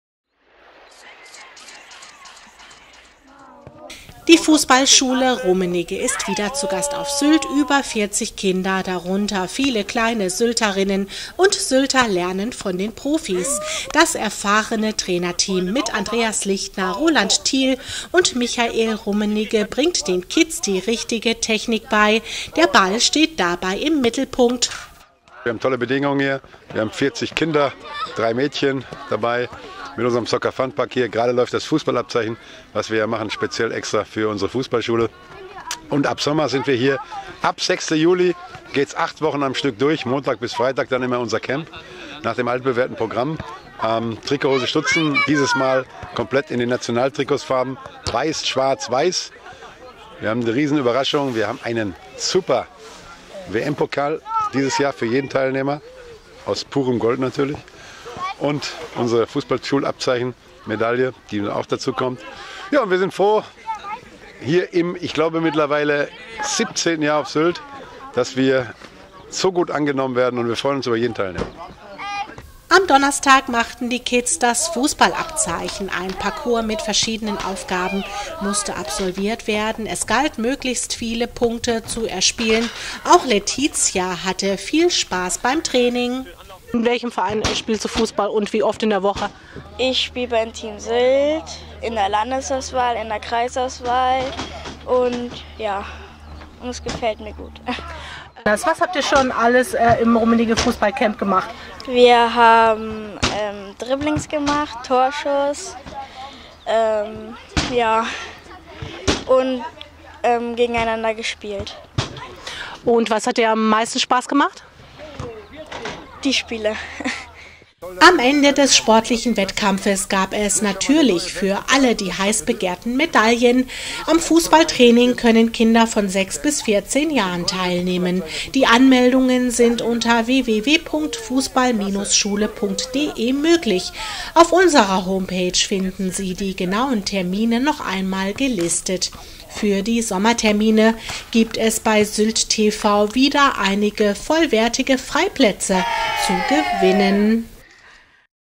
Die Rummenigge Fussballschule gastiert auf Sylt
Die Fussballschule von Michael Rummenigge ist auch in diesem Jahr, für mehrere Fussballcamps in Westerland zu Gast.